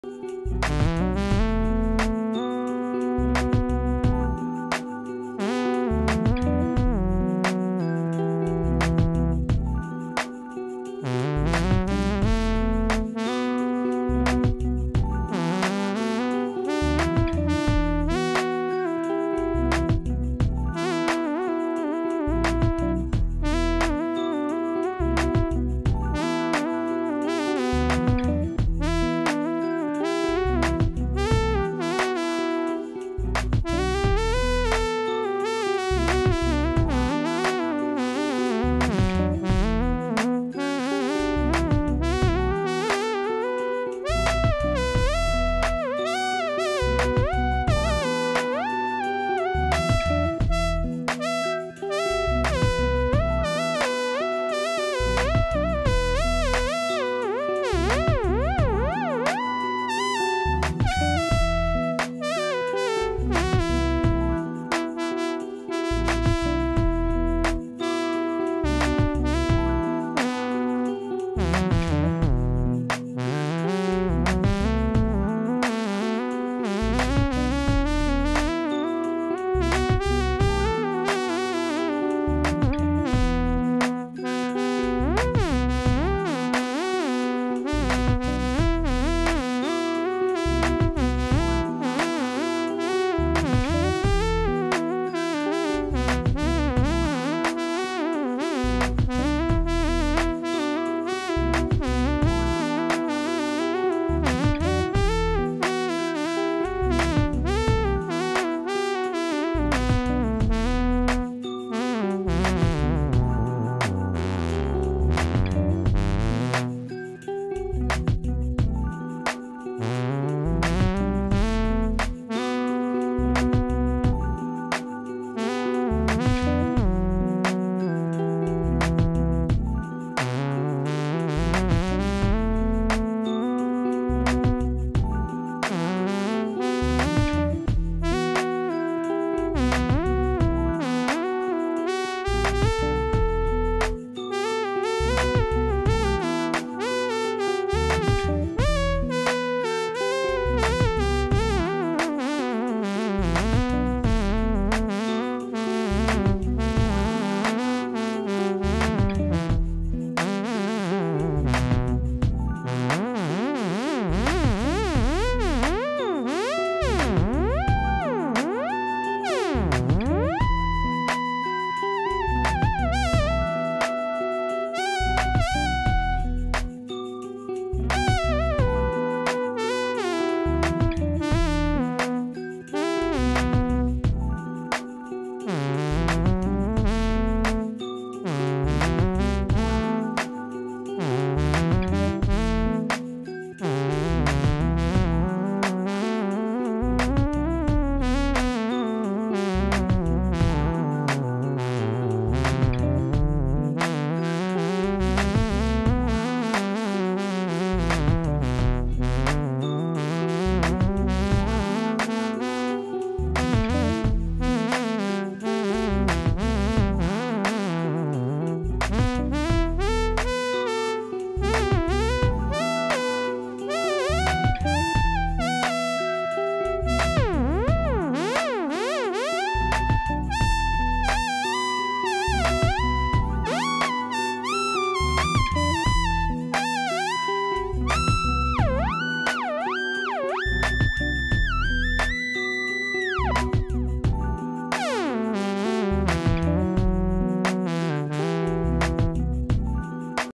SynthFunk | jamuary2023
Instruments
-Moog Sirin  (Lead) Midi to the Roland Keytar
-Roland Ax Edge Keytar (Controller only)
-Mpclive2 (Track)